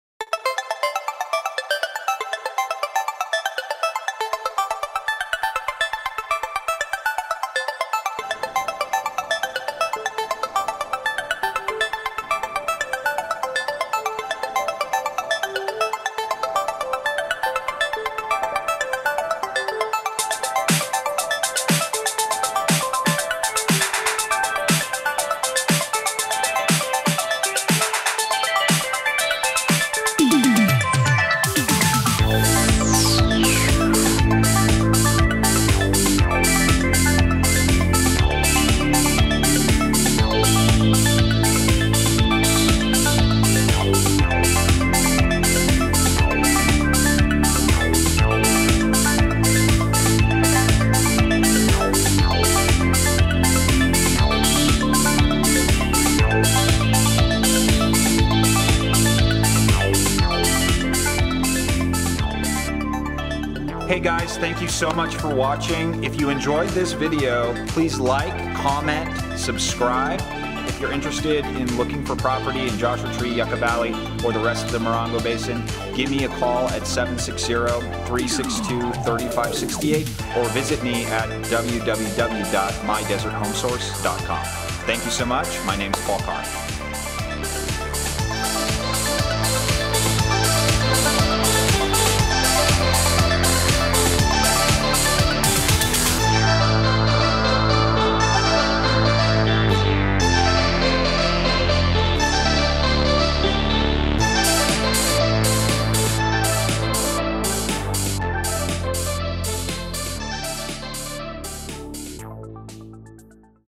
Joshua Tree! Now That's a tiny house! 4918 1st Street $99,900.00
This has got to be the most affordable DEAL in Joshua Tree! Minutes from downtown Joshua Tree and just down the road from the famous biannual Joshua Tree Music Festival Camp Grounds this original desert cabin awaits! The cabin comes equipped with 1 bedroom, 1 full bathroom, living area and bonus room. It's amazing at how much bigger this 400 square foot cabin feels on the inside! Whether you are looking for a place of your own, second home, Airbnb, or investment property this one is truly the best under 100K money can buy! Come see it today before it's gone!